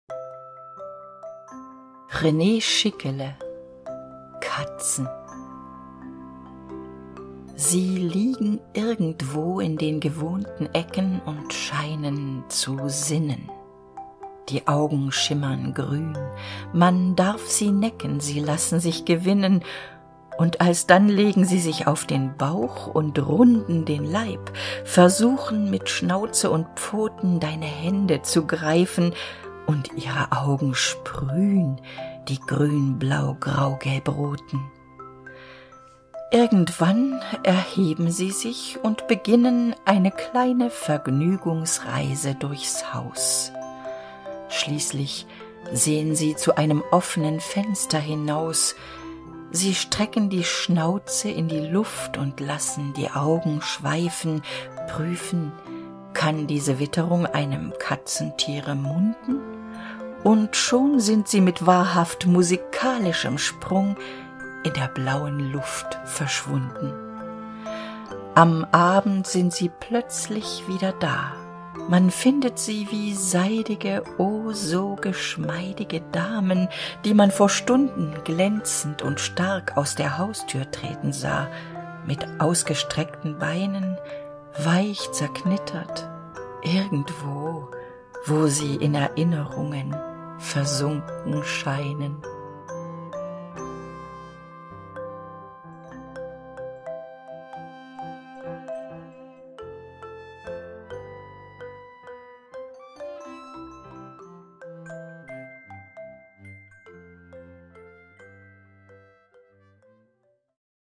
Katzen - Gedicht von René Schickele (1883-1940)
René Schickele: Katzen

Sie liegen irgendwo in den gewohnten Ecken
und scheinen zu sinnen.
Die Augen schimmern grün.
Man darf sie necken,
sie lassen sich gewinnen.
Und alsdann
legen sie sich auf den Bauch und runden
den Leib, versuchen mit Schnauze und Pfoten
deine Hände zu greifen,
und ihre Augen sprühn,
die grünblaugraugelbroten . .
Irgendwann
erheben sie sich und beginnen
eine kleine Vergnügungsreise durchs Haus.
Schließlich sehn sie zu einem offnen Fenster hinaus :
sie strecken
die Schnauze in die Luft und lassen die Augen schweifen,
prüfen: kann diese Witterung
einem Katzentiere munden?
Und schon sind sie mit wahrhaft musikalischem Sprung
in der blauen Luft verschwunden.
Am Abend sind sie plötzlich wieder da.
Man findet sie wie seidige, o so
geschmeidige Damen, die man vor Stunden
glänzend und stark aus der Haustür treten sah,
mit ausgestreckten Beinen
weich zerknittert irgendwo,
wo sie in Erinnerungen versunken scheinen.

Fotos: Ilona Rückert (alle Fotos zeigen den Kater Felix in seinen frühen Entdeckertagen)
Vortrag: Eva K. Kühn

Den Hörkanal gibt es auch als Podcast auf den meisten bekannten Plattformen (Spotify, Amazon Music, Audible, Google Podcasts, ...).